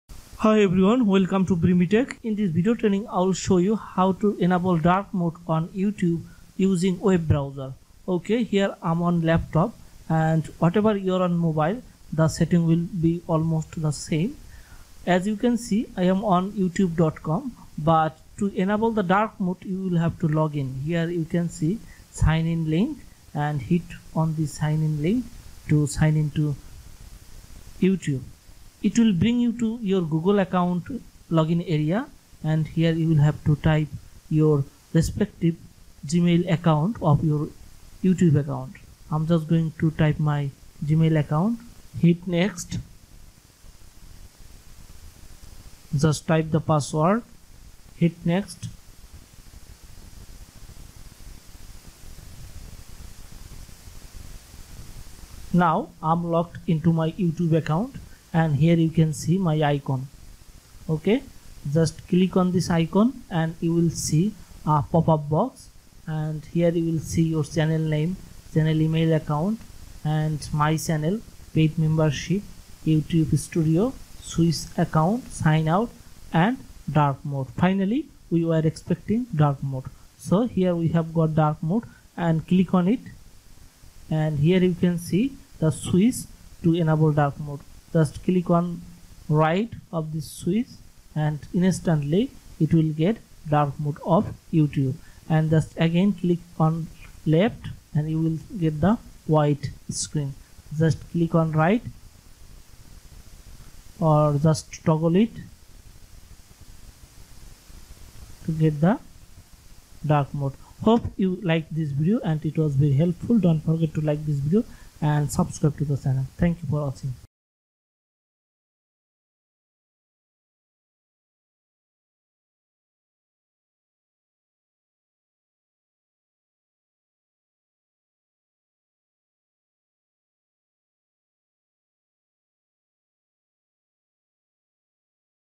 How To Enable YouTube Dark Mode on PC
It is very easy To Enable YouTube Dark Mode .YouTube dark mode will help to safe eye sight specially in the darkened room. The standard default mode creates much bright light that may hamper eyes in the long run. In 2017 YouTube dark mode was first introduced for web browser . Later in 2018 it was available for Android and iOS .